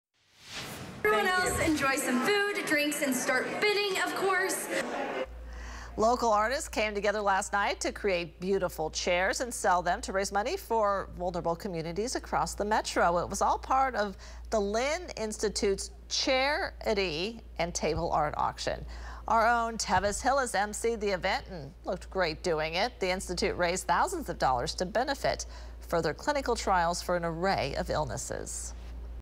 Art Chair Auction Benefits Clinical Trials
Local artists came together Friday night to create beautiful chairs and sell them to raise money for vulnerable communities across the metro.